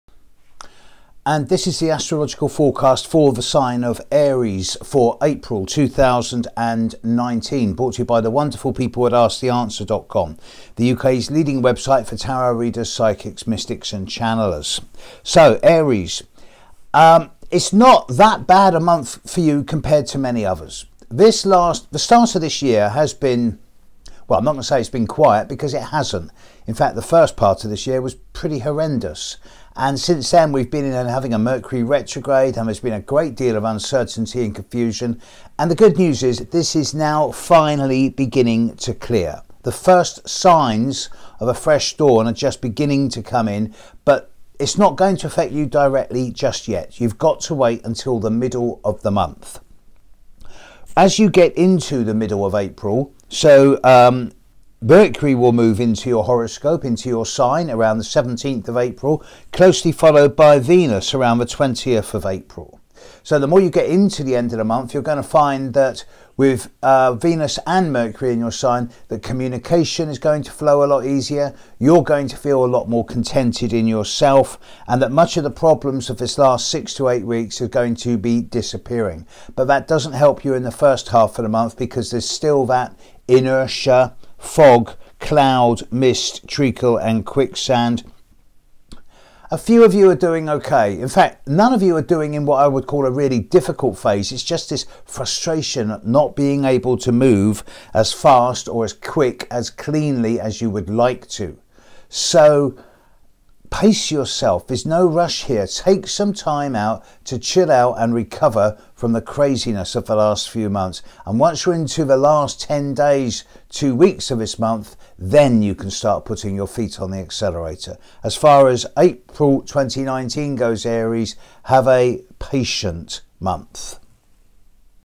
Aries Horoscope - April 2019
AskTheAnswer.com brings you our FREE monthly videoscopes by our resident astrologer Steve Judd. In this video Steve Judd gives his astrological forecast for Aries in April 2019.